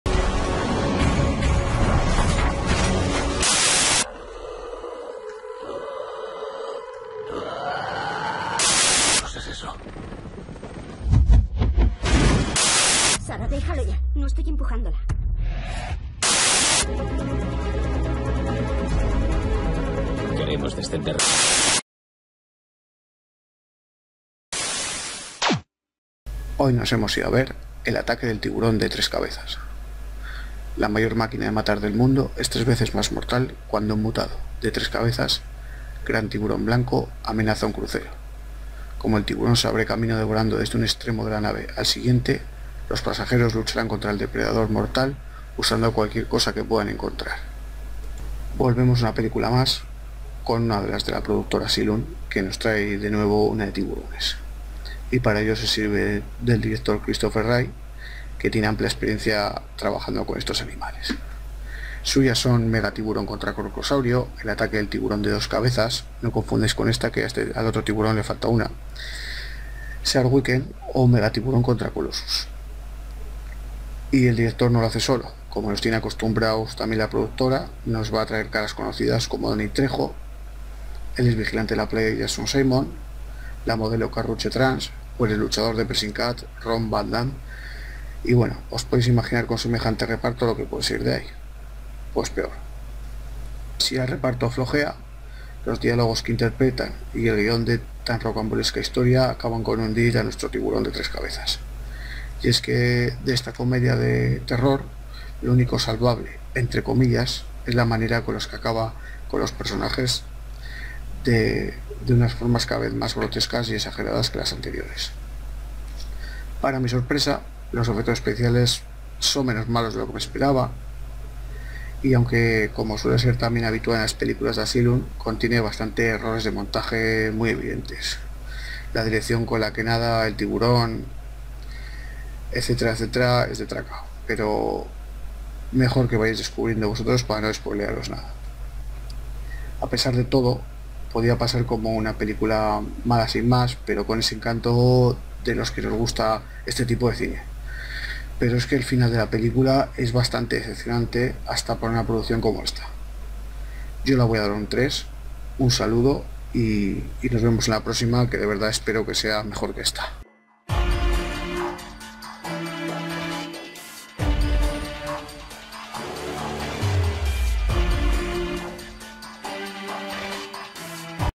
Videocrítica El ataque del tiburón de tres cabezas (sin spoilers)
Título original: 3 Headed Shark Attack (Three Headed Shark Attack).
Año:2015.
País: Estados Unidos.
Director: Christopher Ray.
Guión: Jacob Cooney, Bill Hanstock.
Fotografía:Alexander Yellen.
Productora: Asylum.
Reparto: Jena Sims, Danny Trejo, Karrueche Tran, Jaason Simmons, Kimberly Battista, Dawn Hamil, Brianna Ferris, Rob Van Dam, Brad Mills, Rebekah Hart Franklin, Sharon Posey, Cristalle C. Leonard.
Género:Terror, Ciencia ficción,Serie B.